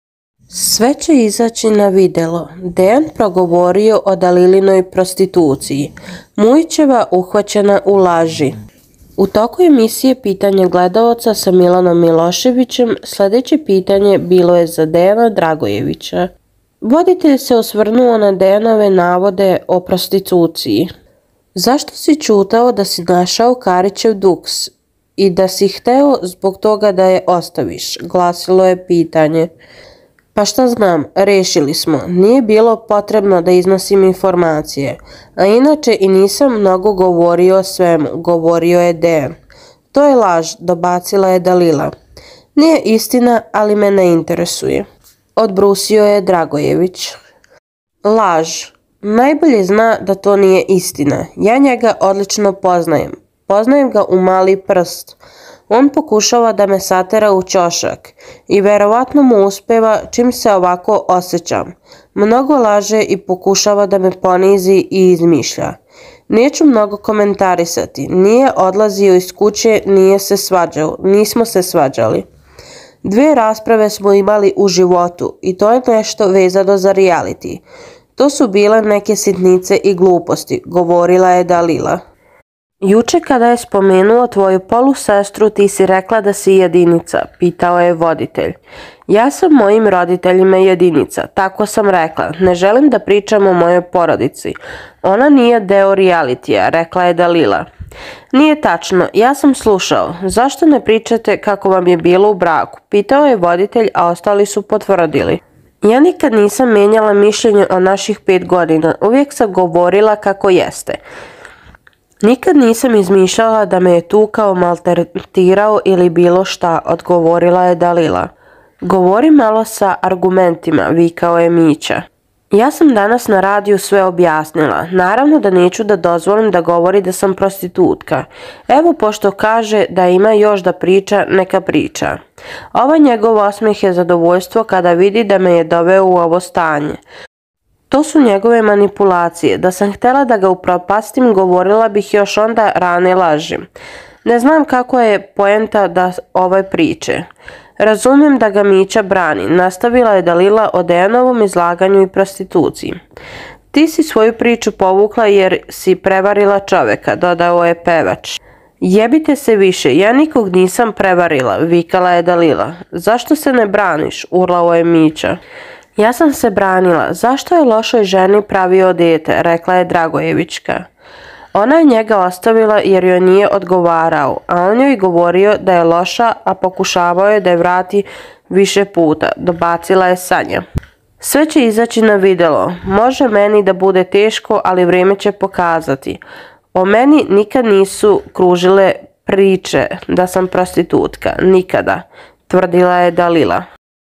SVE ĆE IZAĆI NA VIDELO Dejan progovorio o Dalilinoj pros***uciji, Mujićeva uhvaćena u laži
Laže! Najbolje zna da to nije istina. Ja njega odlično poznajem, poznajem ga u mali prst... On pokušava da me satera u ćošak i verovatno mu uspeva čim se ovako osećam. Mnogo laže i pokušava da me ponizi, izmišlja... Neću mnogo komentarisati... Nije odlazio iz kuće, nije se svađao, nismo se svađali. Dve rasprave smo imali u životu i to ništa vezano za rijaliti. To su bile neke sitnice i gluposti... - govorila je Dalila